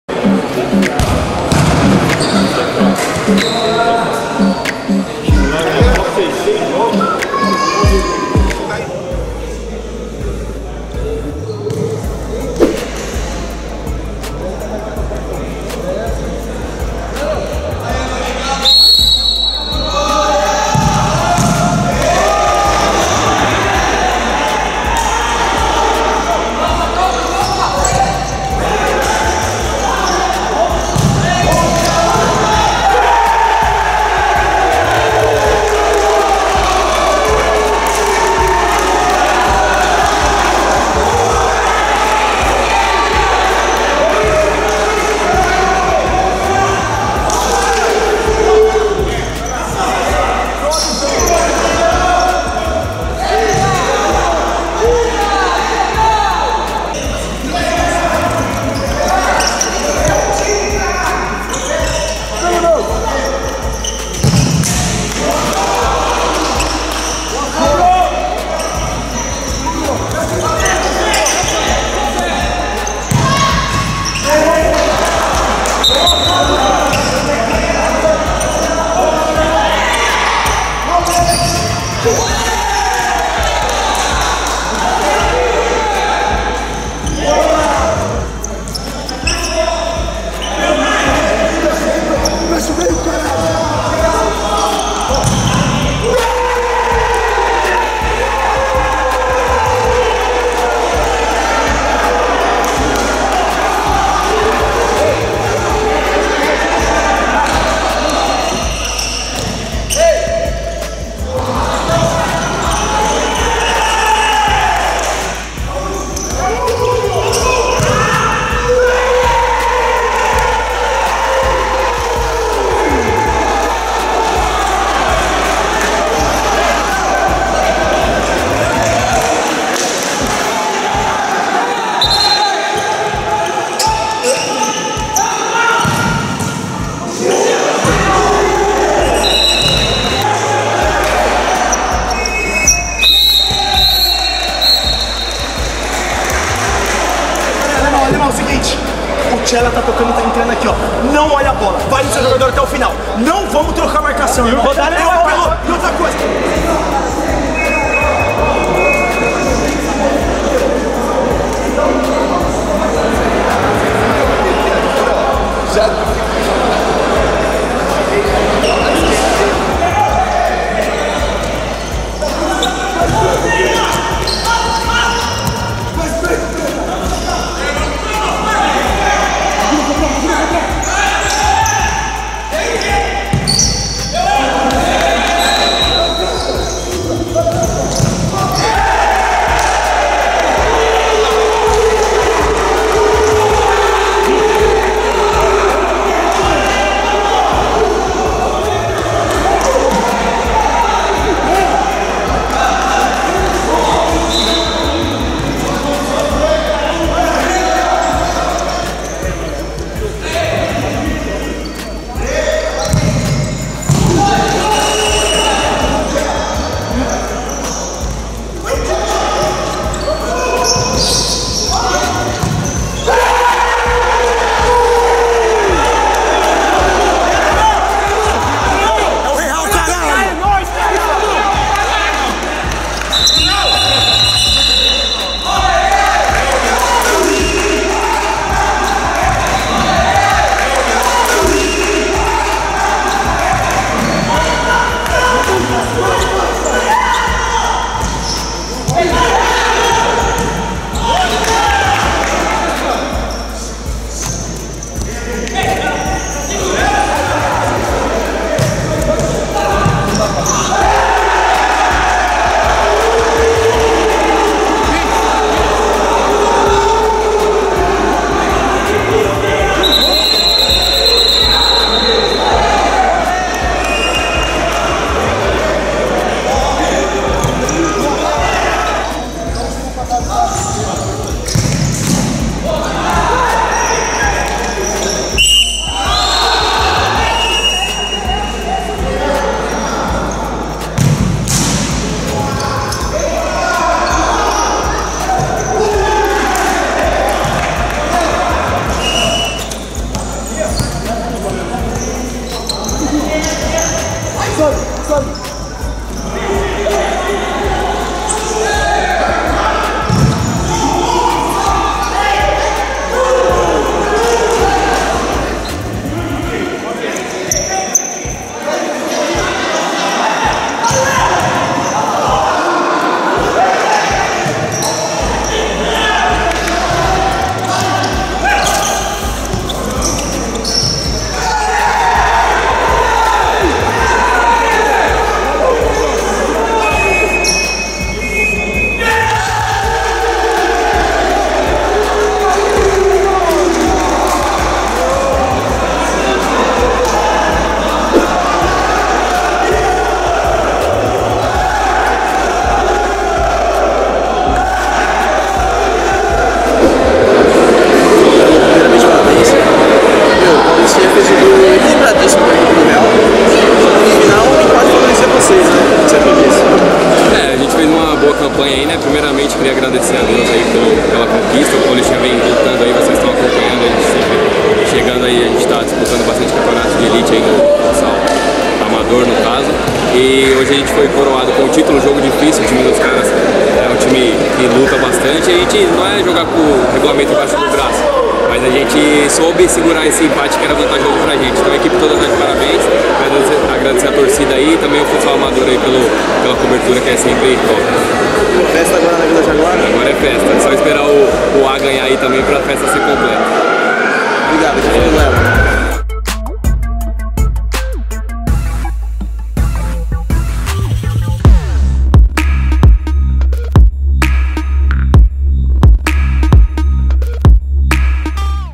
Paulistinha x Real Freguesia - Final Copa Alvorada 2018
JOGO REAL: FINAL COPA ALVORADA 2018

Confira os melhores momentos Paulistinha Vila Jaguara e Real Futsal da Freguesia do Ó, pela fiinal da Copa Alvorada de Futsal 2018. A partida foi disputada no domingo 25 de novembro no ginásio do Metal Clube, no bairro Ayrosa em Osasco.